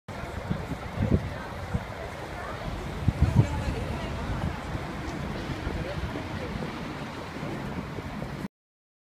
Longest quew ever at Brussels Central Station, Belgium 2016
Soliers and policmen checking travellers bags.
Impressive and worrying